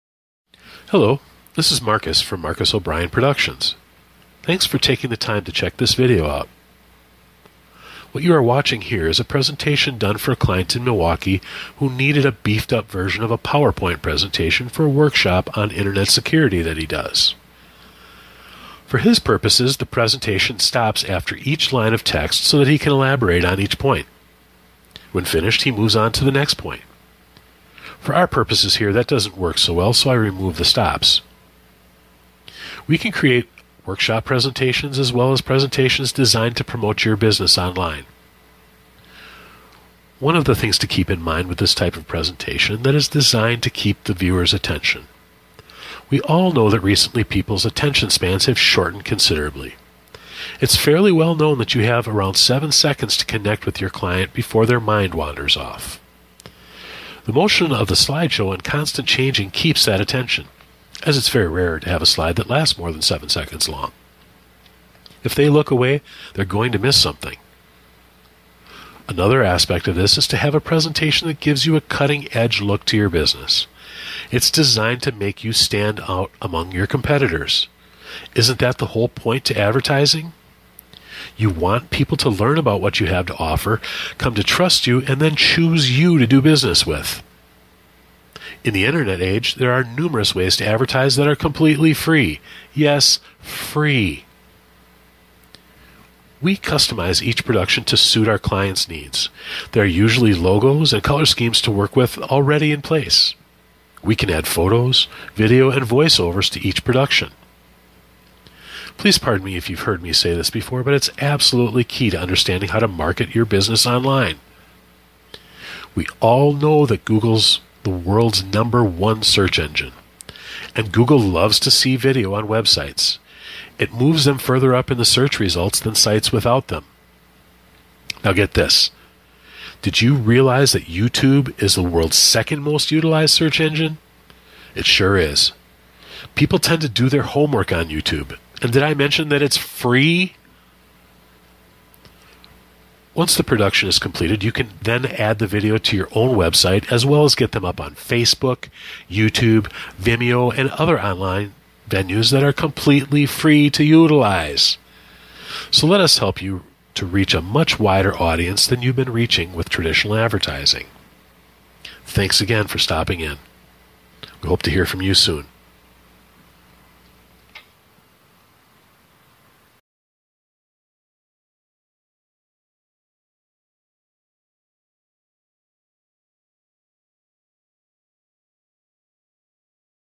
Presentation for Right Click-Computer Services for their workshop on Internet Security
This is an amped up version of a PowerPoint presentation created for Right-Click Computer Services in Milwaukee for a workshop they do on Internet Security.

The voice over explains what Marcus O'Brian Productions does as the slideshow is somewhat separate. It is only meant to be a visual presentation.